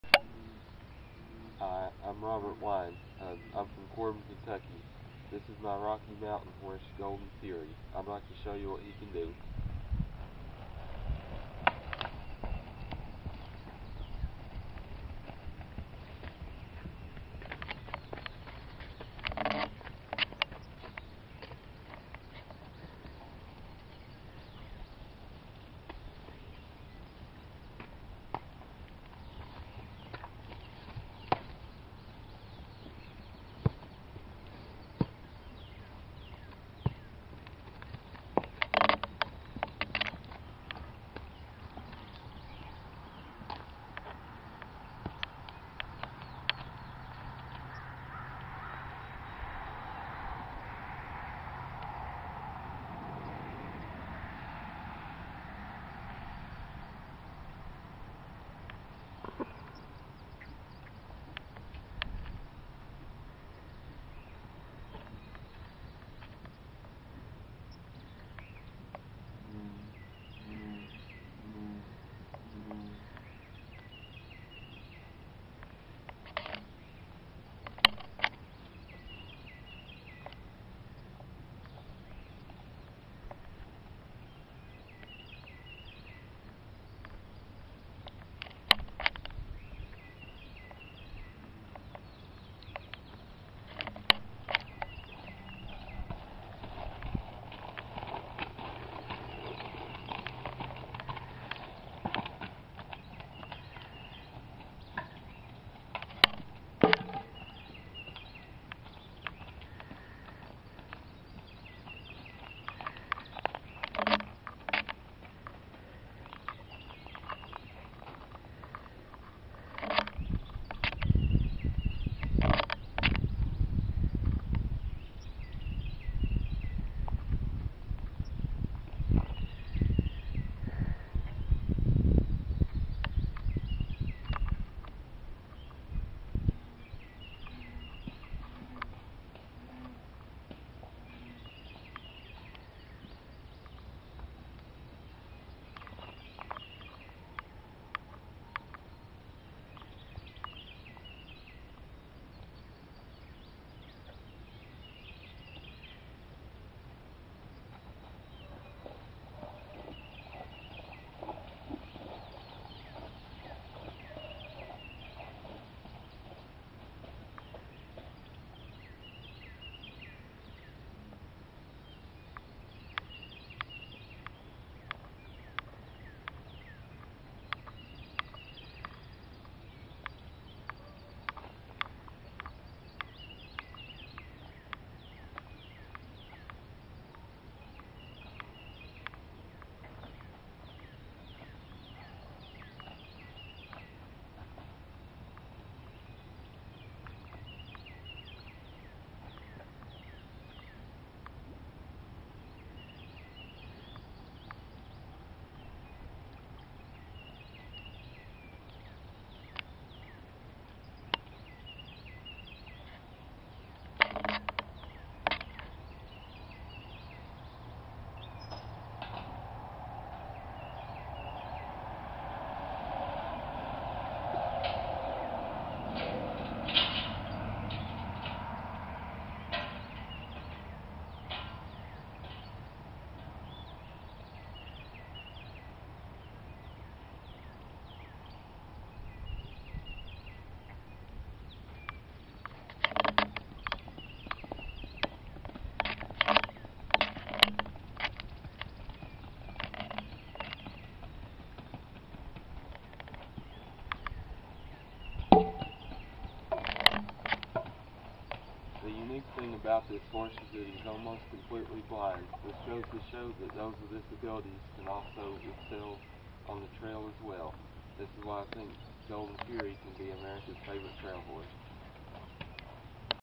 AFTH Pleasure Division - Golden Fury
This is Golden Fury, a 10 year old Rocky Mountain Gelding. He is special because he is almost blind from Uveitis (Moon Blindness). He is still a great trail horse! He competed in his first ACTHA ride on April 28, 12 at Mammoth Cave Nat. Park. He didn't win, but he successfully completed the course even scoring some 9's and 10's. He also went on a couple of 5 hour trail rides thru rough terrain and did fine. He is learning some voice commands like, 'Step', to step over a log or onto the trailer. He is very trustworthy and will do whatever you ask.
Some of Golden Fury's history: We bought Fury 5 years ago as a Stallion. He also had a large knot covered with proud flesh (scar tissue) on the outside of his rear leg above the fetlock. I successfully treated this and removed the proud flesh. He still has a hard knot that is visible but doesn't cause him any trouble.
Fury bred 2 of our mares in 2009 but the foals did not make it. One of them only lived 3 days. The other was born to a pony that we had to sell. After the foal was born and weaned, the owner sold the foal, a filly, back to us. Fury was 'Momma' to his own baby. He loved her and would take care of her. This filly lived for 7 months and then passed away. Fury grieved over her for a few days but eventually got over it. We decided to geld Fury because we didn't want this to happen again. It took about a year for Fury to adjust his attitude, but it was worth it. He earned the nickname, 'The Bear', because he could be a 'Grizzly Bear' at times as a stallion, but as a gelding he is now a big, 'Teddy Bear'.
Fury started having episodes of 'Moon Blindness' about 2 years ago. He is now almost totally blind, but that doesn't stop him. I have had people ask me, 'Are you going to put him down?' I say, "No Way!". He is still a great ride and we think he can be America's Favorite Trail Horse.